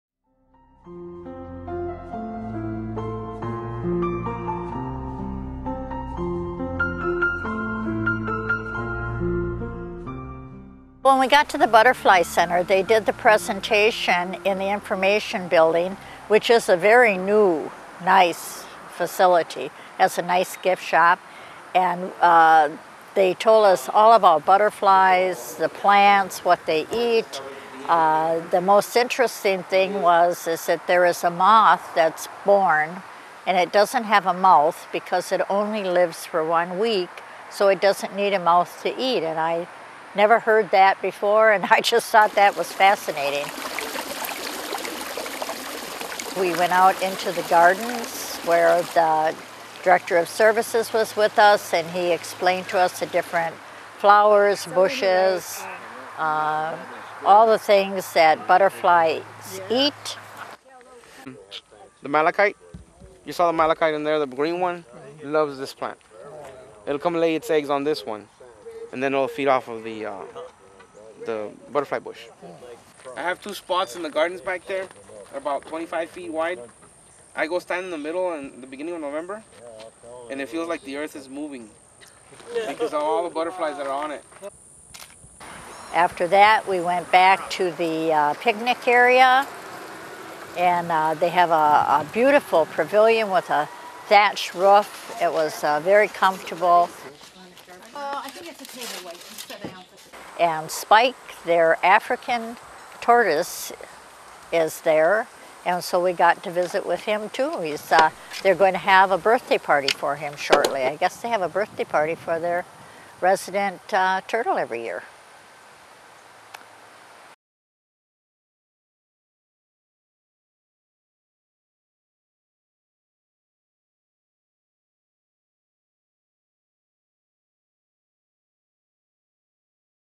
What I learned visiting the National Butterfly Center Mission, Texas
What I learned visiting the National Butterfly Center Mission, Texas in December.

OleanderAcres.com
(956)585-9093